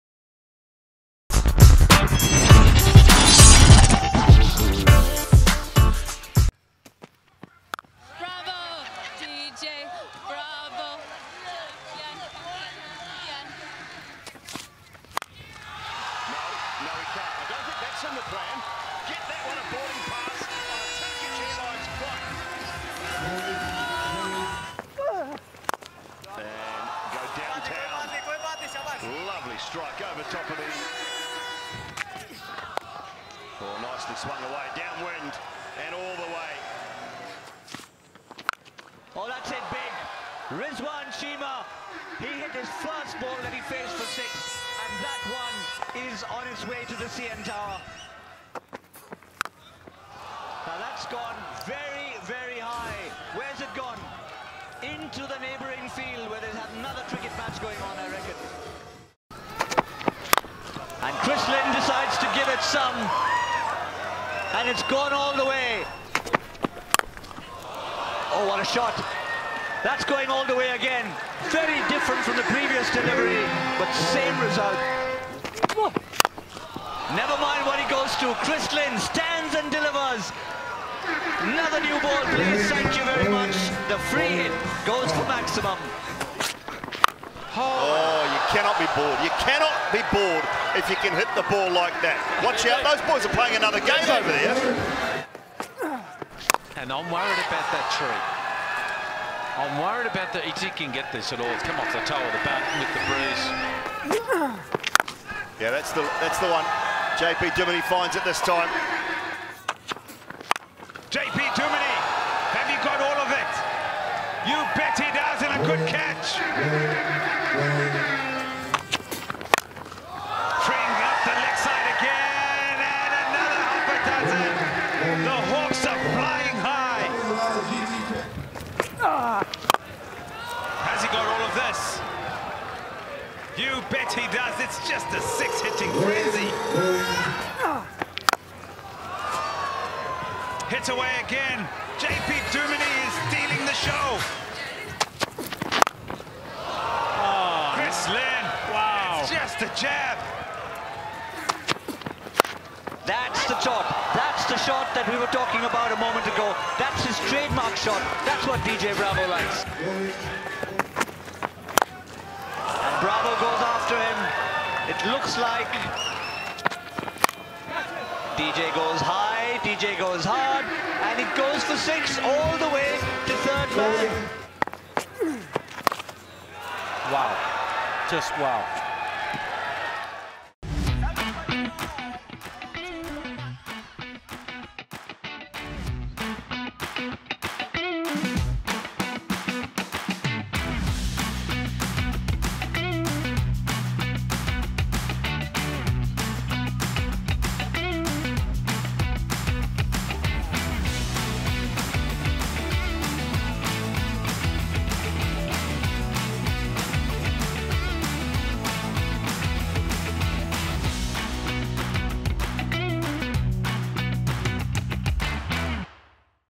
Super sixes | Vancouver Knights vs Winnipeg Hawks | Match 5 Highlights | GT20 Canada 2019
Watch full highlights of’s match at Brampton Canada, match 5 of the global T20 (GT20) Canada 2019.
For more action.

Vancouver Knights vs  Winnipeg Hawks  | Match 5 Highlights | GT20 Canada 2019